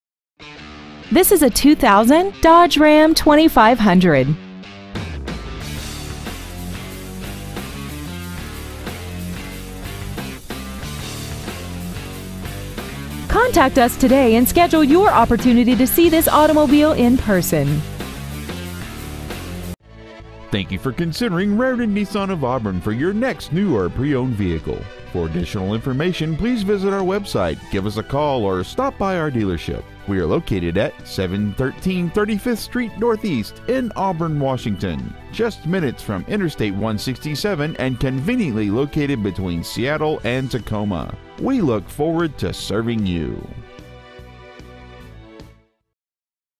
Preowned 2000 Dodge Ram 2500 Auburn WA 98002
Year : 2000
 Make : Dodge
 Model : Ram 2500
 Engine : 5.9L I6 Turbo Diesel

 Exterior : White
 Miles : 65,001

 Stock : YM235529